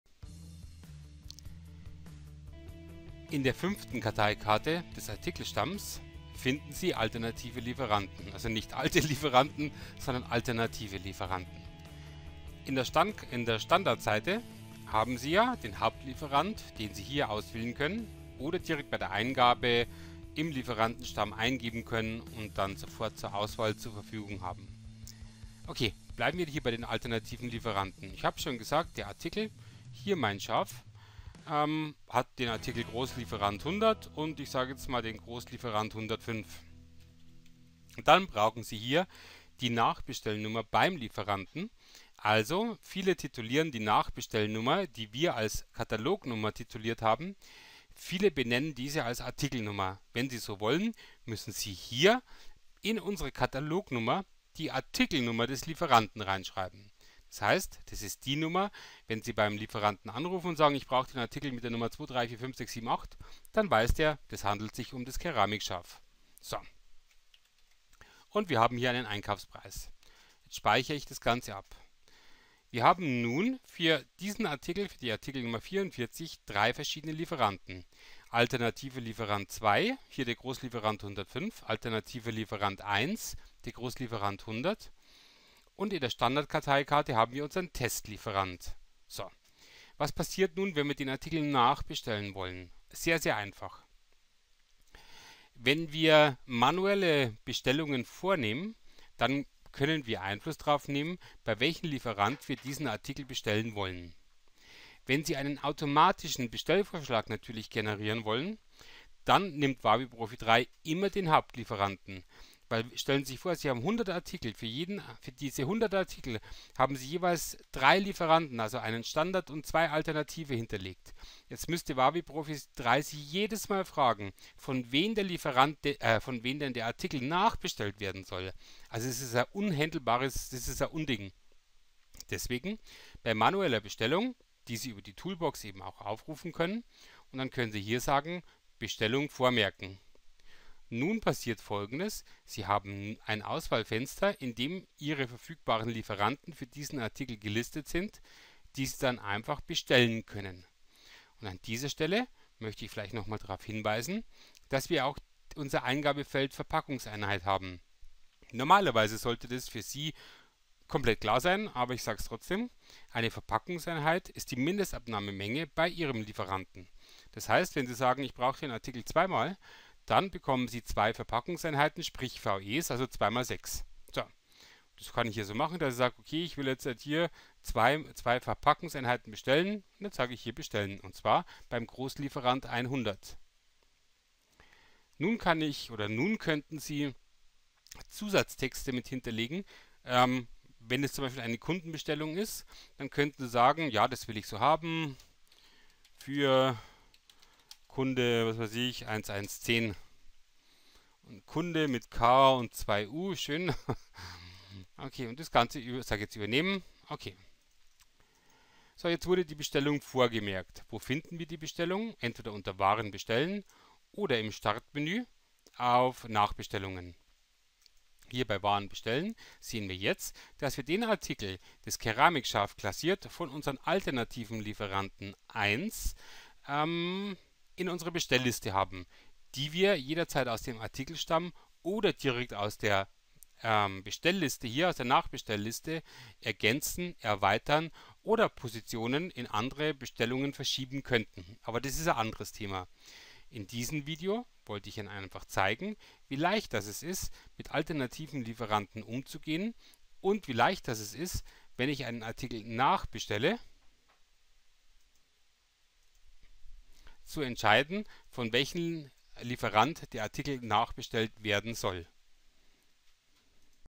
WaWi-Profi 3.x, Artikelstamm, alternative Lieferanten und Nachbestellungen
Alternative Lieferanten und Nachbestellungen im Artikelstamm.